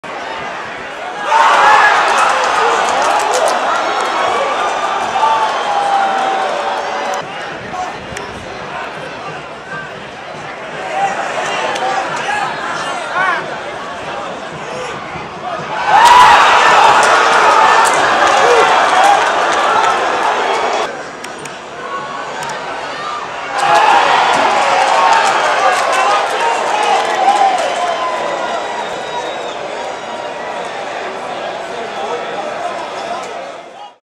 GOLS - Anápolis 1 x 3 Goiás - Goianão 2016
Carlos Eduardo (2) e Rafhael Lucas marcaram os gols da vitória esmeraldina por 3 a 1 sobre o Anápolis, no Jonas Duarte, pelo Goianão 2016.